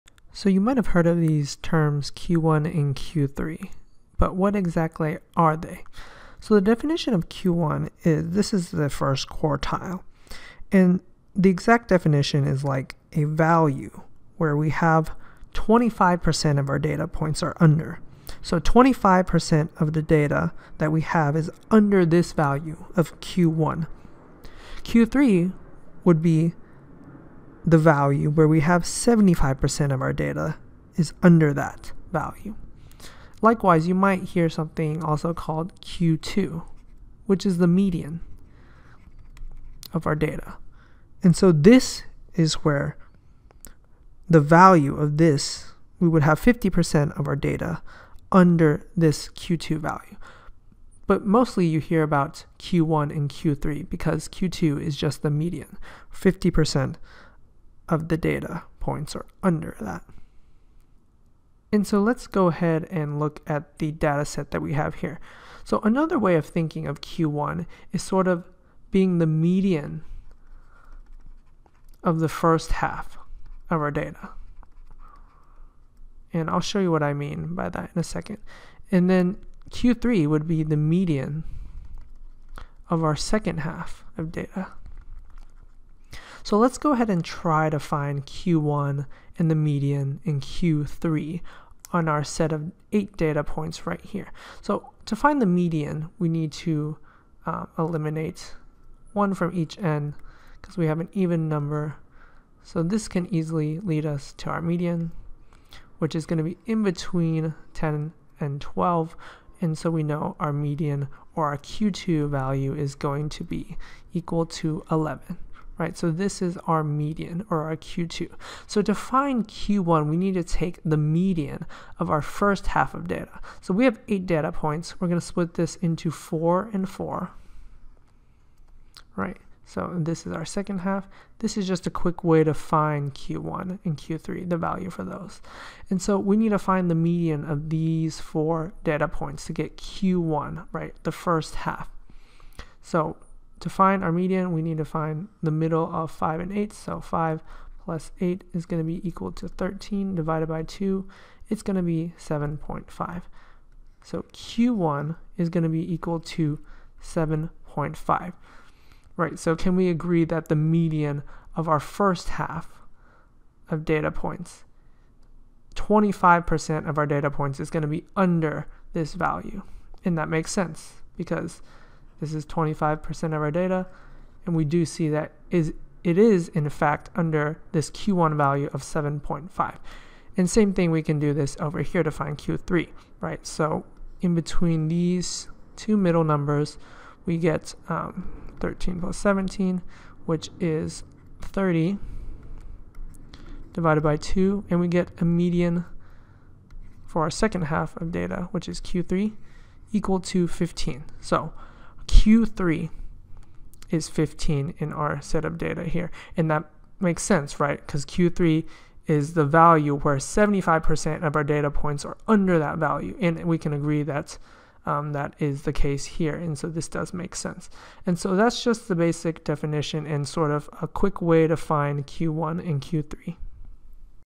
The definition of Q1 and Q3 and how to calculate them from a set of data points
This video explains both the definition of Q1 and Q3 and a quick way to find them from a set of data points. We also talk about Q2 or the median. Q1 and Q3 are the first and third quartile representing the value where 25% of our data is under and 75% of our data is under, respectively. Q1 can be thought of as the median of our first half of data and Q3 can be though of as the median of our second half of data.